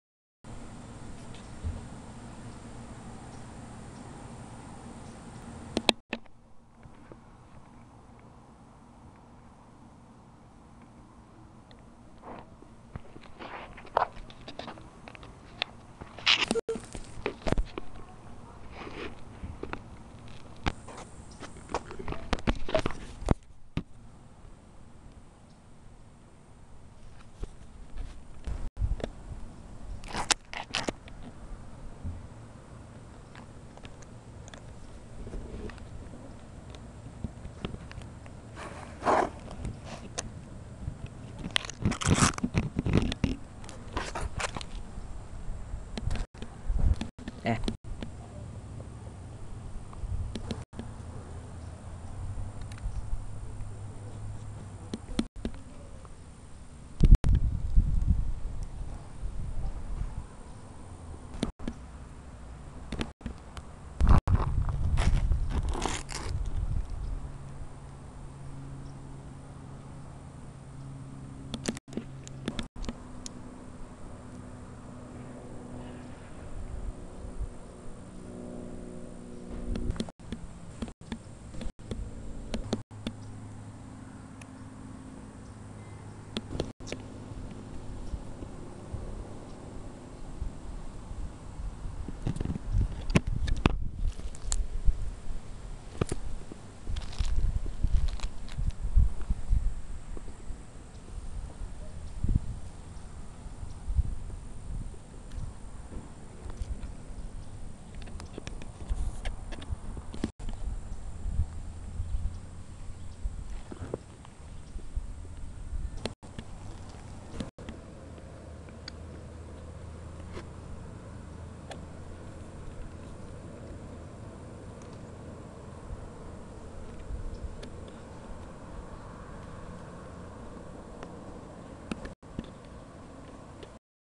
My brown widow (Latrodectus geometricus).
Just a video of me playing with my brown widow. If only more people would realize how fascinating a spider can be.